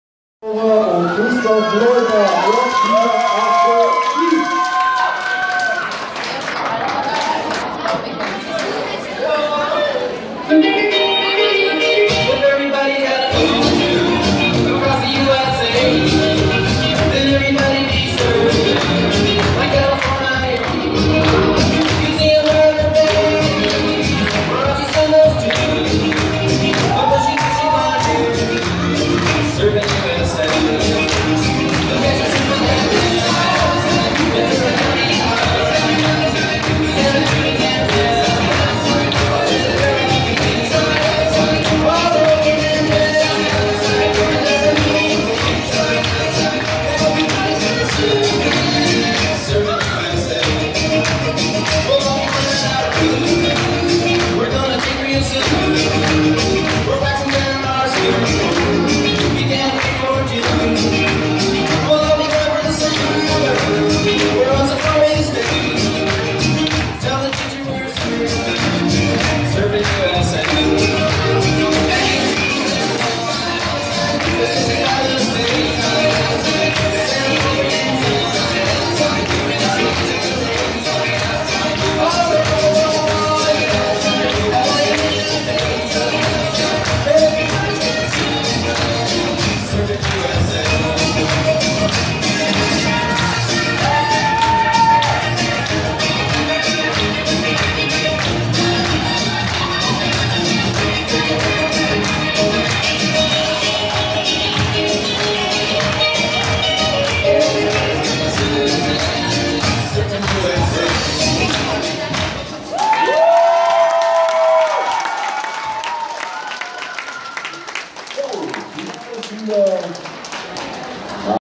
Acrobatic Rock'n'Roll Juveniles Final, BT Poysdorf 22-10-2016
Christoph - Cynthia,, RRC Rockfever ASKÖ Wien, 4. Place